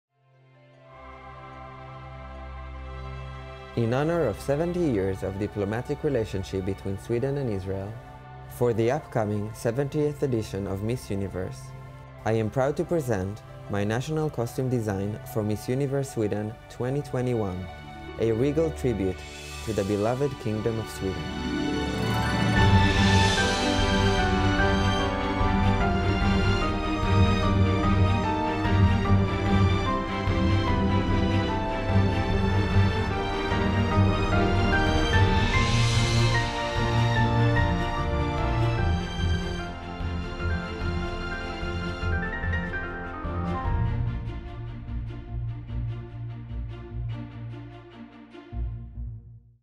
National Costume for Miss Universe Sweden 2021
Proud to present Aviad Arik Herman's National Costume design for Her Majesty Miss Universe Sweden 2021 Moa Sandberg

This National Costume is a Regal tribute to the beloved Royal Kingdom of Sweden, in honour of 70  years of diplomatic relationship between Sweden & Israel, and the 70th edition of the Miss Universe competition!

Concept & Design: Aviad Arik Herman
Design Assistant: Sarit Herman
Photography: Peter Håkansson
Make Up: Emanuel Hareb
Hair: Tara Ahmed
Videography: Shir Musan
Special thanks to Gunnebo Castle for the magical location